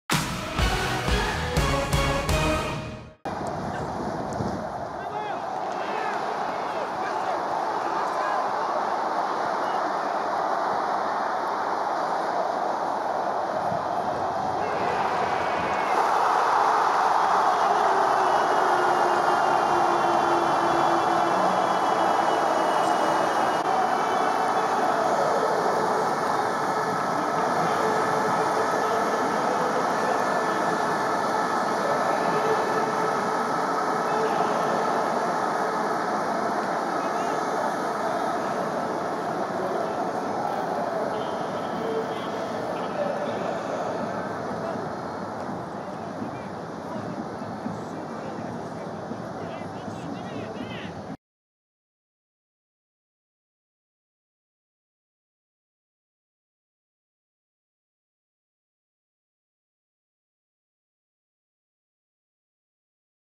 Goal Rémi OUDIN (82' - BORDEAUX) FC GIRONDINS DE BORDEAUX - NÎMES OLYMPIQUE (2-0) 20/21
"FC GIRONDINS DE BORDEAUX - NÎMES OLYMPIQUE (2-0) Rémi OUDIN (82')  goal. FC GIRONDINS DE BORDEAUX vs NÎMES OLYMPIQUE Goals in video

Ligue 1 Uber Eats - Season 2020/2021 - Week 8 MATMUT ATLANTIQUE - Sunday 25 October 2020